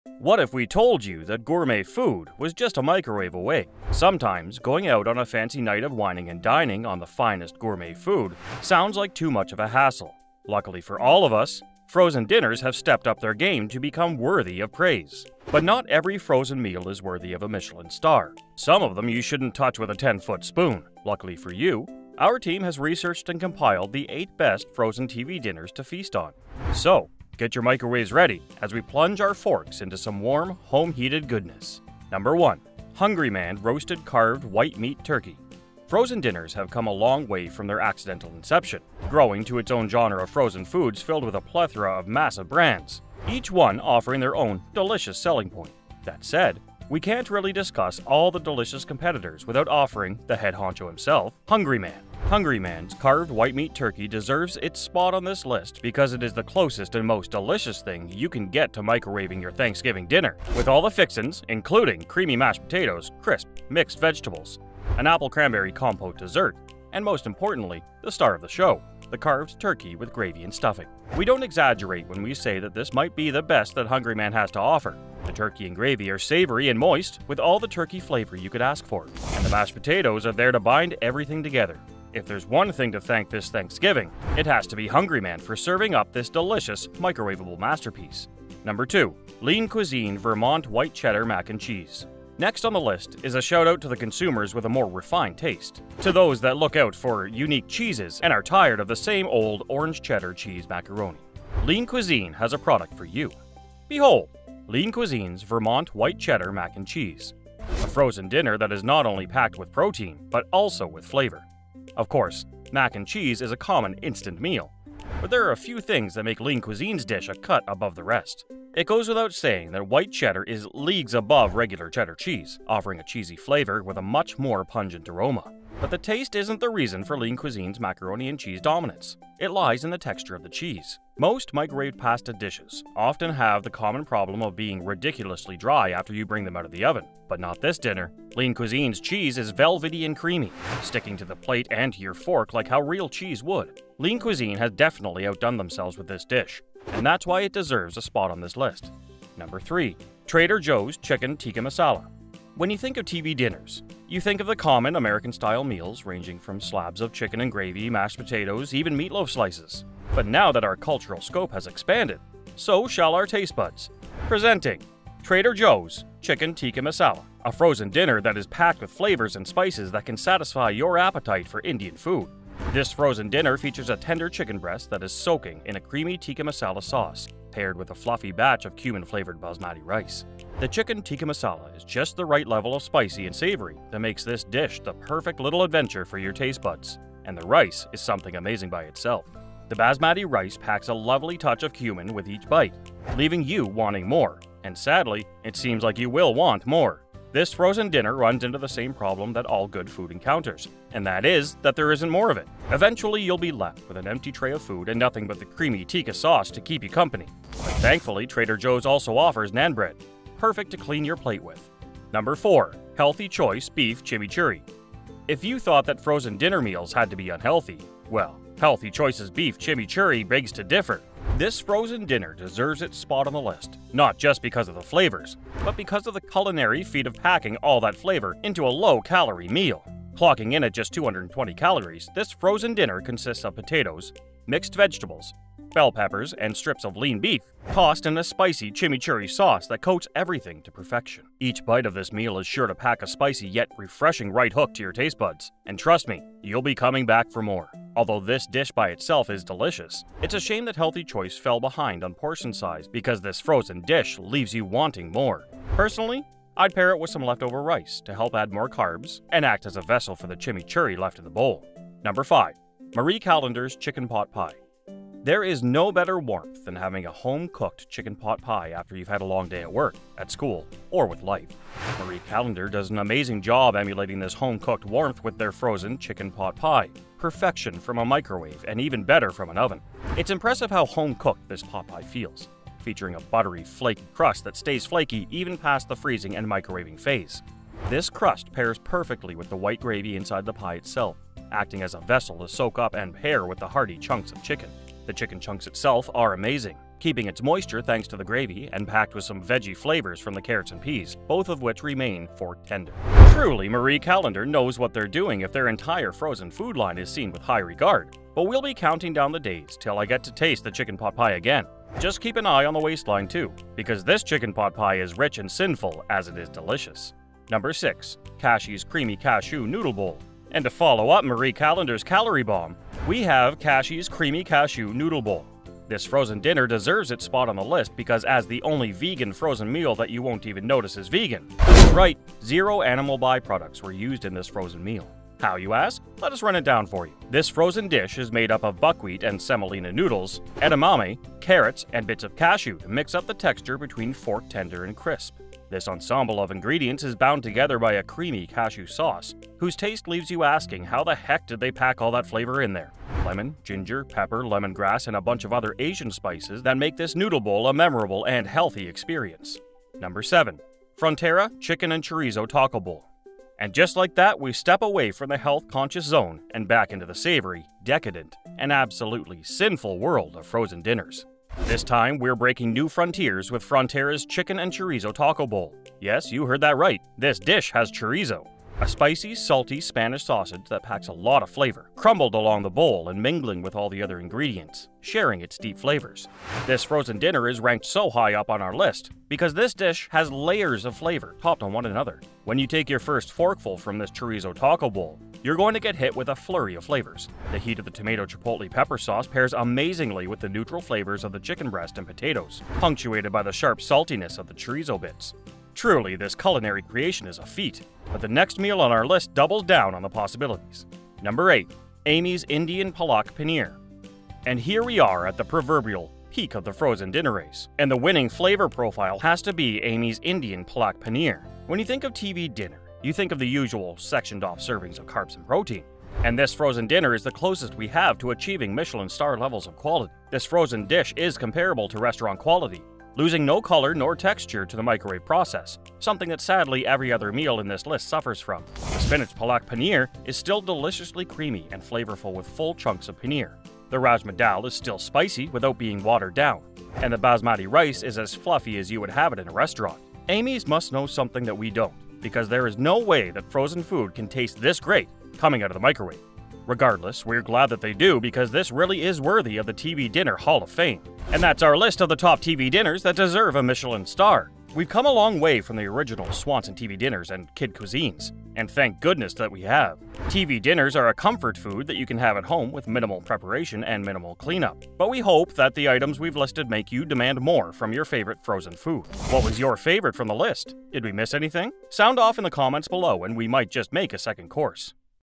TV Dinners That Are Worth A Michelin Star
TV Dinners That Taste Like Michelin Starred Food!

Check out our top 10 TV dinners that make the cut for delicious alternatives to dining out or homecooked food!

In today's video we look at TV Dinners That Taste Like Michelin Starred Food!

Inspired by The Delicious History of TV Dinners

Inspired by How TV Dinners Changed The Way America Cooked, Forever

Inspired by Frozen Food Ads vs. Real Life Food (Test)

Inspired by The Rise and Fall of the TV Dinner - Cheddar Explains